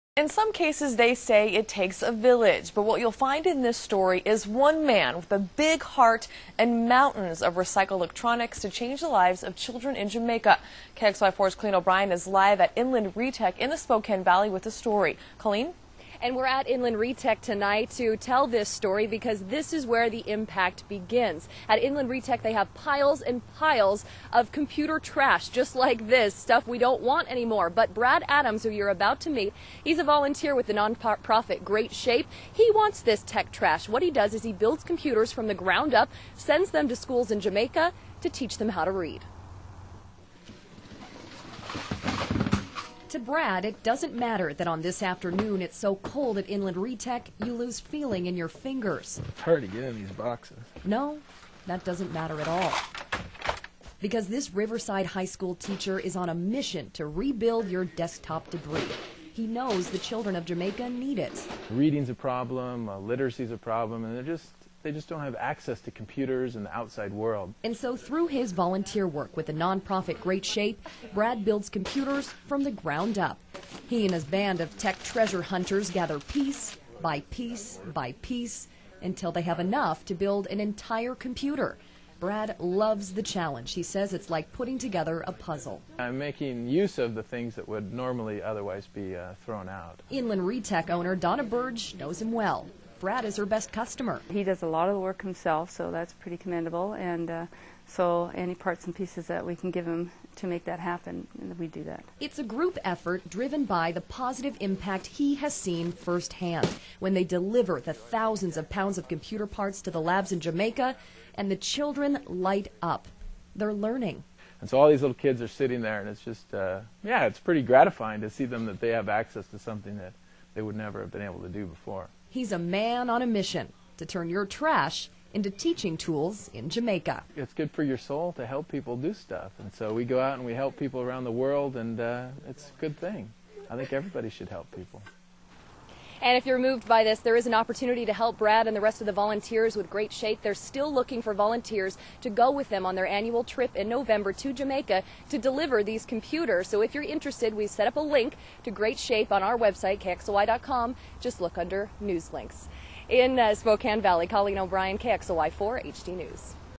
Your trash is this man's treasure
In most cases the saying "it takes a village" rings true but to bring literacy to children in Jamaica it takes one man with a big heart and mountains of recycled electronics. KXLY4's Colleen O'Brien has more.